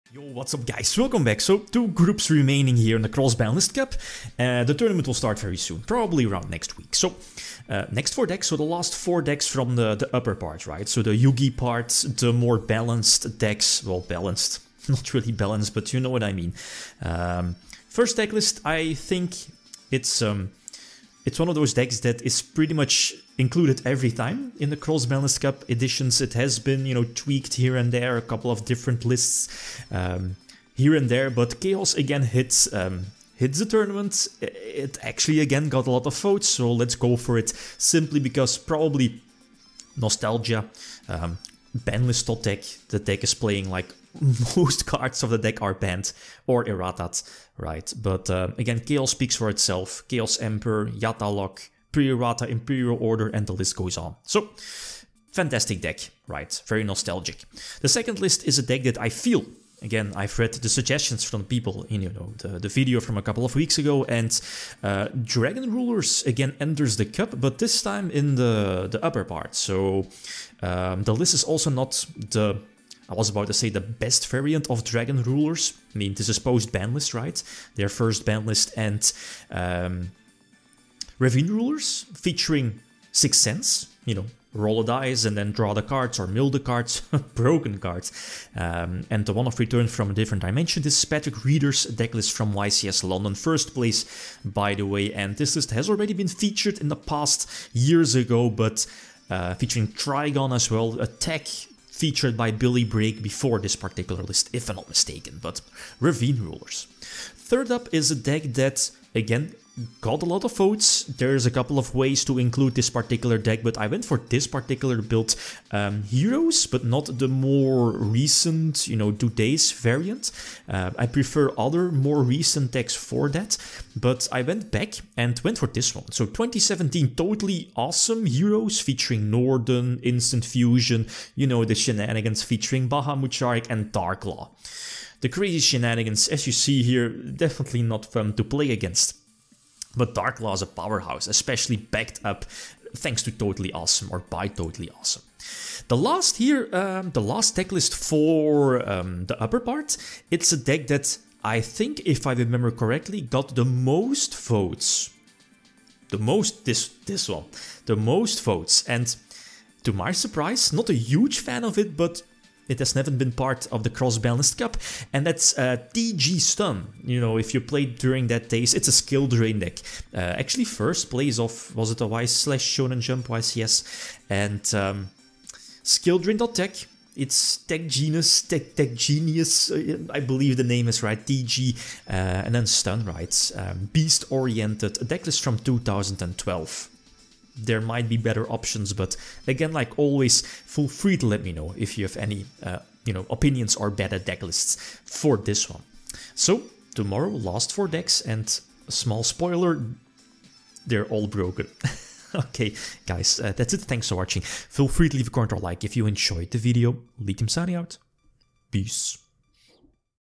Cross-Banlist Cup 2022 | Group D
Presenting the 7th group for the 2022 Cross-Banlist Cup! Changes? Let me know!